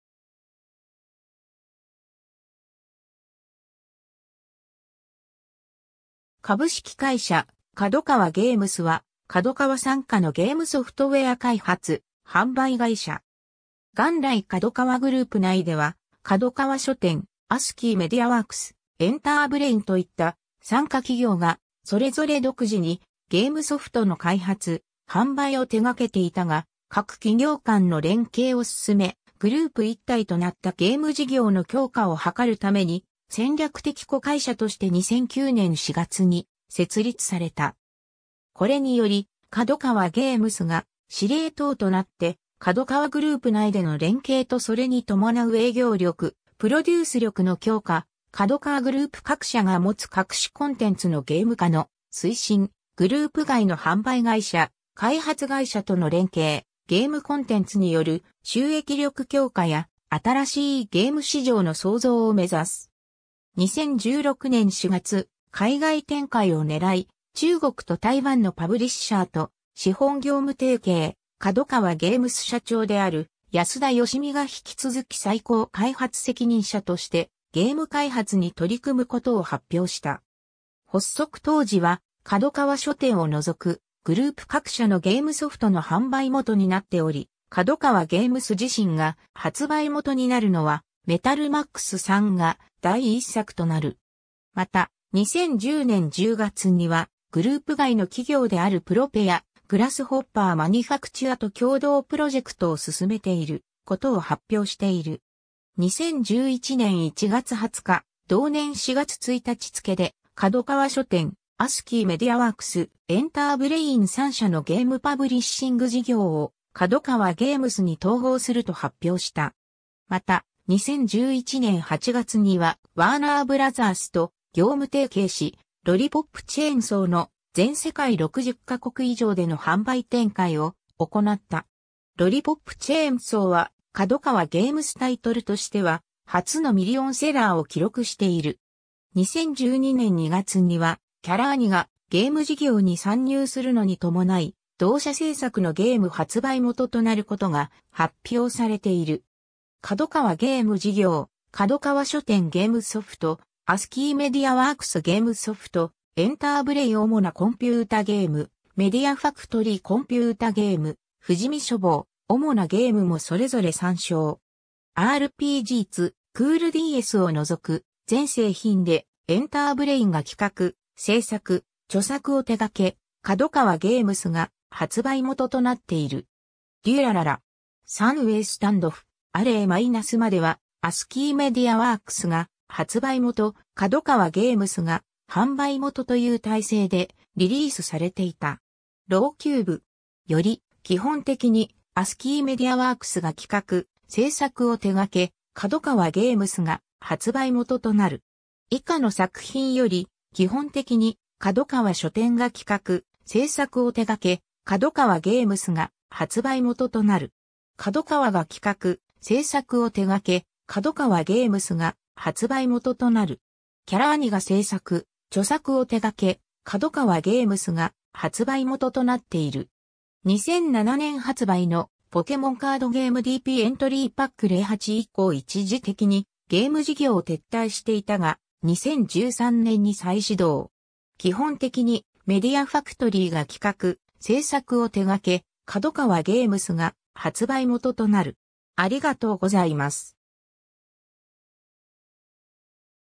角川ゲームス
日本のコンピュータゲームメーカー・ブランド
株式会社角川ゲームス（かどかわゲームス）は、KADOKAWA傘下のゲームソフトウェア開発・販売会社。
 元来角川グループ内では、角川書店、アスキー・メディアワークス、エンターブレインといった傘下企業が、それぞれ独自にゲームソフトの開発・販売を手がけていたが、各企業間の連携を進め、グループ一体となったゲーム事業の強化を図るために、戦略的子会社として2009年4月に設立された。
これにより、角川ゲームスが司令塔となって、角川グループ内での連携とそれに伴う営業力・プロデュース力の強化、角川グループ各社が持つ各種コンテンツのゲーム化の推進、グループ外の販売会社・開発会社との連携、ゲームコンテンツによる収益力強化や新しいゲーム市場の創造を目指す。
 2016年4月、海外展開を狙い中国と台湾のパブリッシャーと資本業務提携、角川ゲームス社長である安田善巳が引き続き最高開発責任者としてゲーム開発に取り組むことを発表した。
 発足当時は角川書店を除くグループ各社のゲームソフトの販売元になっており、角川ゲームス自身が発売元になるのは『メタルマックス3』が第1作となる。
また、2010年10月にはグループ外の企業であるプロペやグラスホッパー・マニファクチュアと共同プロジェクトを進めていることを発表している。
2011年1月20日、同年4月1日付で角川書店、アスキー・メディアワークス、エンターブレイン3社のゲームパブリッシング事業を角川ゲームスに統合すると発表した。
また、2011年8月にはワーナー・ブラザースと業務提携し、ロリポップチェーンソーの全世界60ヶ国以上での販売展開を行った。
ロリポップチェーンソーは、角川ゲームスタイトルとしては初のミリオンセラーを記録している。
2012年2月にはキャラアニがゲーム事業に参入するのに伴い、同社製作のゲーム発売元となることが発表されている。
 KADOKAWA#ゲーム事業、角川書店#ゲームソフト、アスキー・メディアワークス#ゲームソフト、エンターブレイン#主なコンピュータゲーム、メディアファクトリー#コンピュータゲーム、富士見書房#主なゲームもそれぞれ参照。
 RPGツクールDSを除く全製品で、エンターブレインが企画・制作・著作をてがけ、角川ゲームスが発売元となっている。
 『デュラララ!! 3way standoff -alley-』まではアスキー・メディアワークスが発売元、角川ゲームスが販売元という体制でリリースされていた。
『ロウきゅーぶ!』より基本的にアスキー・メディアワークスが企画・制作を手がけ、角川ゲームスが発売元となる。
 以下の作品より基本的に角川書店が企画・制作を手がけ、角川ゲームスが発売元となる。
 KADOKAWAが企画・制作を手がけ、角川ゲームスが発売元となる。
 キャラアニが制作・著作をてがけ、角川ゲームスが発売元となっている。
 2007年発売の『ポケモンカードゲームDP エントリーパック'08』以降一時的にゲーム事業撤退していたが、2013年に再始動。
基本的にメディアファクトリーが企画・制作を手がけ、角川ゲームスが発売元となる。